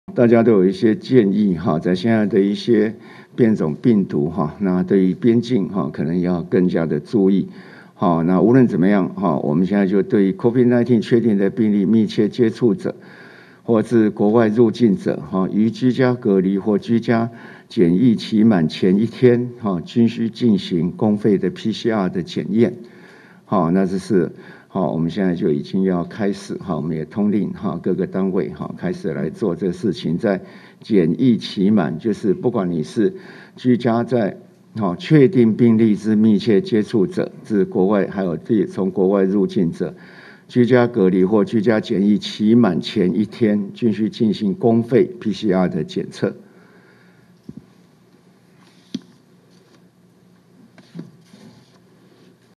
防範印度變種病毒 居檢、居隔「期滿前1天採檢」【健康資訊】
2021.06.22【健康資訊】我國疫情指揮中心今（22）公布，為防範印度Delta變種病毒，即日起無論有無症狀，只要是與新冠確診者密切接觸者、自國外入境者，皆須於居家隔離或居家檢疫期滿前一天，進行公費PCR檢測。